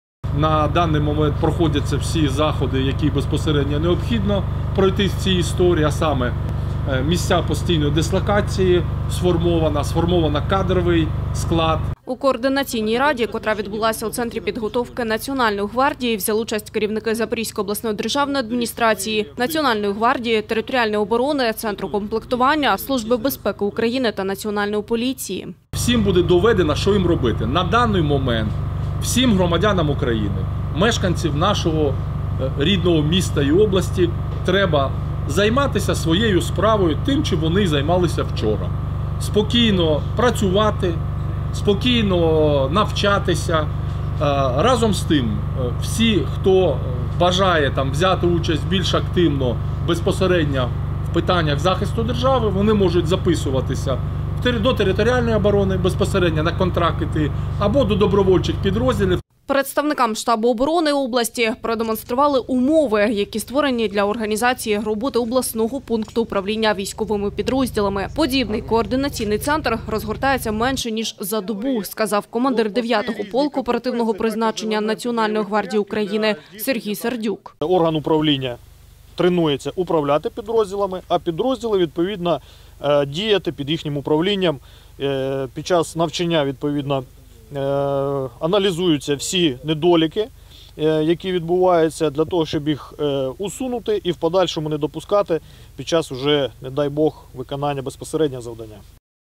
Голова Запорізької ОДА розповів про готовність сил тероборони та області в цілому | Новини
Територіальна оборона Запорізької області сформована на 75%, а ситуація у регіоні контрольована. Про це Суспільному повідомив голова Запорізької державної адміністрації Олександр Старух під час робочої поїздки представників штабу оборони області. За його словами, силами територіальної оборони укомплектована 110 окрема бригада, що складається з шести батальйонів.